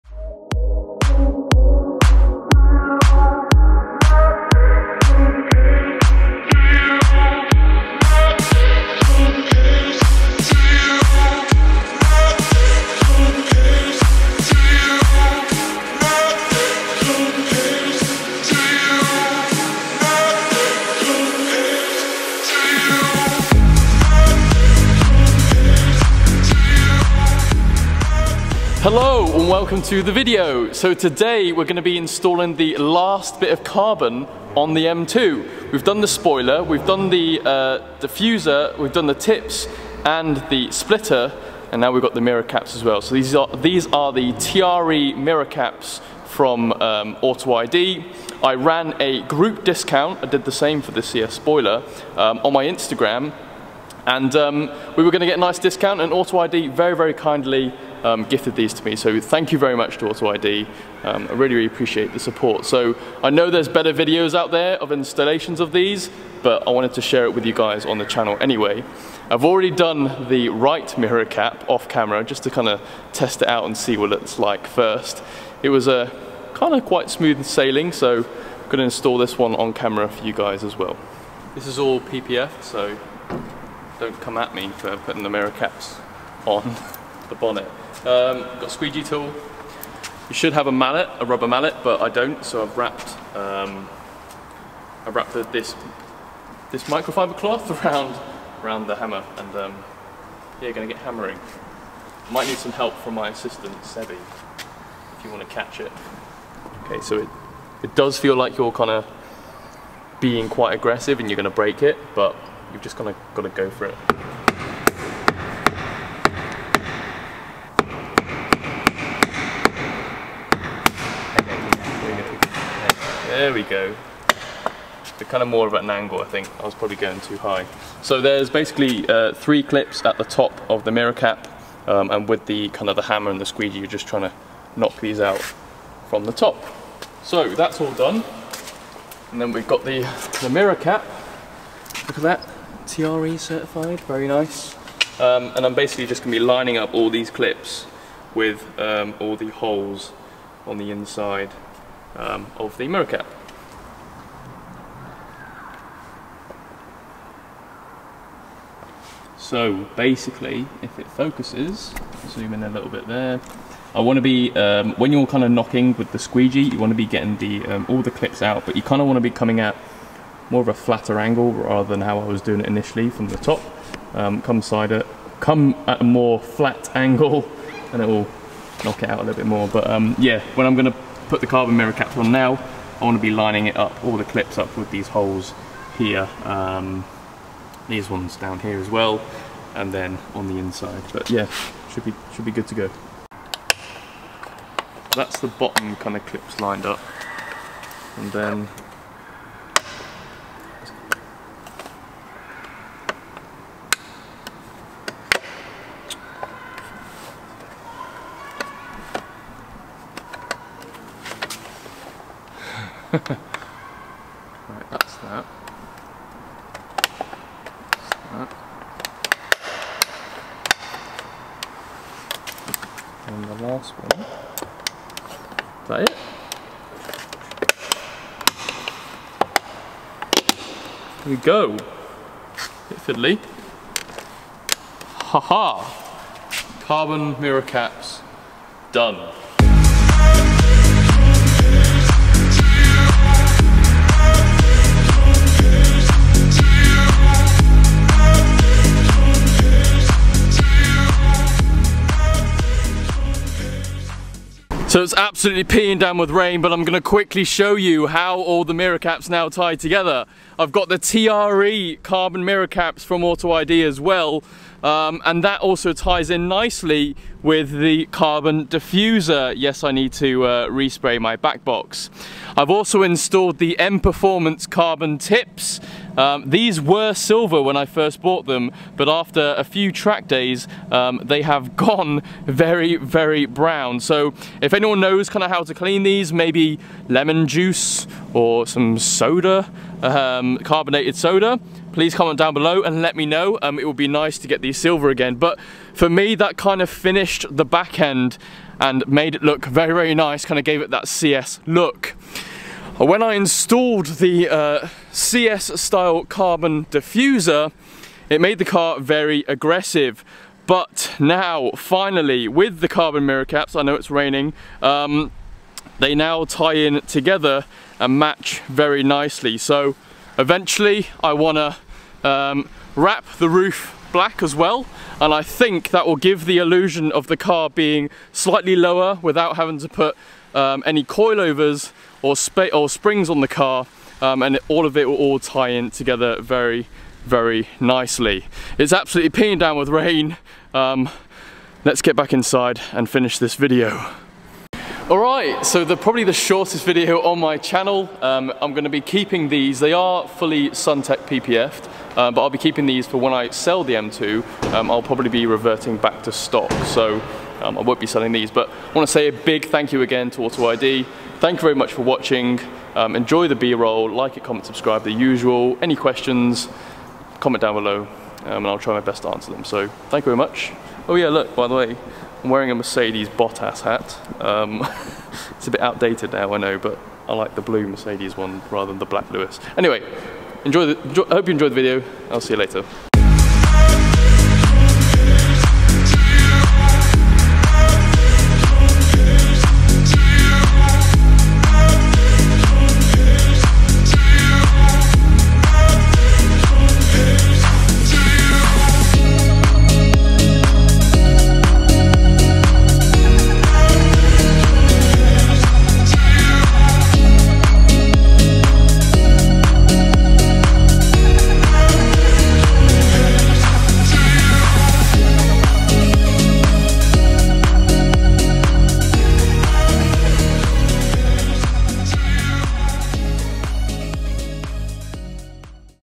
How To Install M2 Competition Mirror Caps!
0:00 Intro
1:21 Mirror Cap Removal
2:24 Mirror Cap Installation
3:59 THE FINAL LOOK!
5:44 Summary